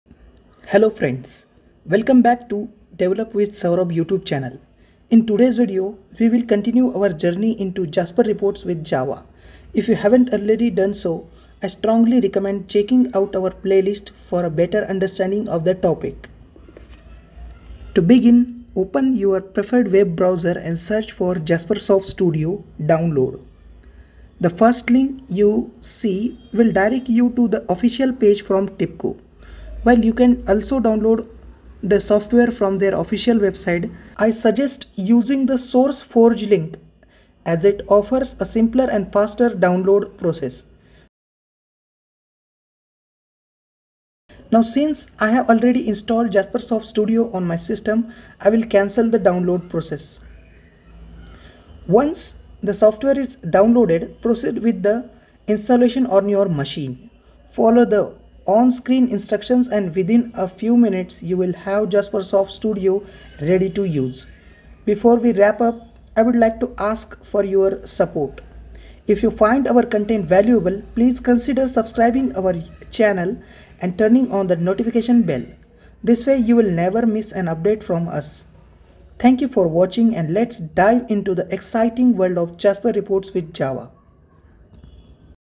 Jasper Reports with Java Tutorial Part 2 | JasperSoft Studio Installation Guide
Welcome to Part 2 of our Jasper Reports with Java tutorial series! In this video, we will guide you step-by-step on how to install JasperSoft Studio, a powerful reporting tool, on your system. JasperSoft Studio is an essential tool for designing, creating, and editing Jasper reports efficiently.

Whether you're a beginner or an experienced developer, this installation guide will provide you with a solid foundation to start leveraging the capabilities of JasperSoft Studio. We'll walk you through the entire installation process, ensuring that you have the necessary components and configurations to seamlessly integrate JasperSoft Studio into your workflow.

By the end of this video, you'll have a fully functional JasperSoft Studio environment ready to create stunning and dynamic reports with Java. So, let's dive in and get started with Part 2 of our comprehensive Jasper Reports with Java tutorial series.